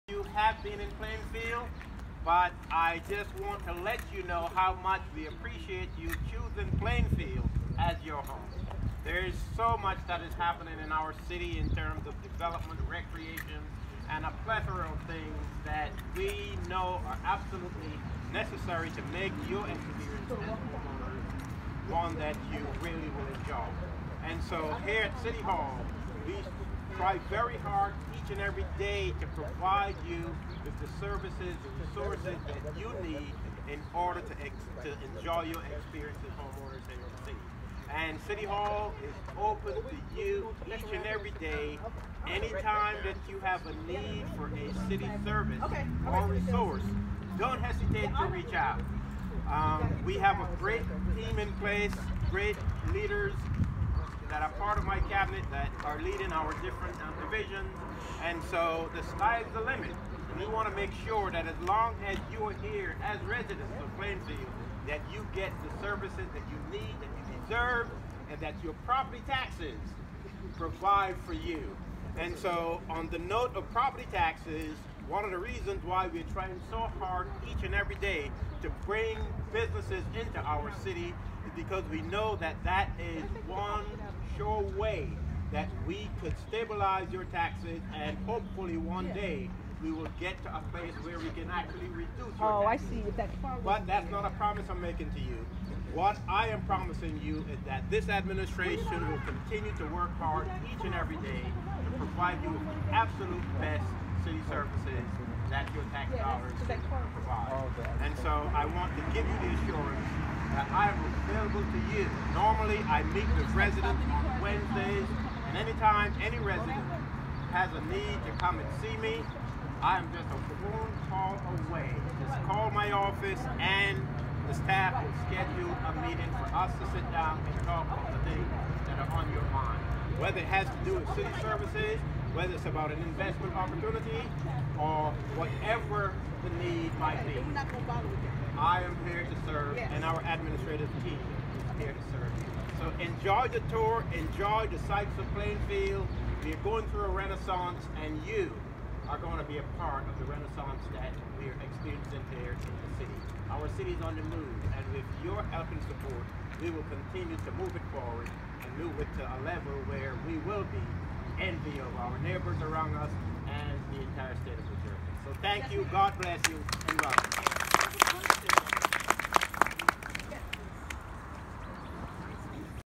Mayor Mapp talks to new residents before their tour begins
A new residents tour was run by the Division of Parks and Recreation.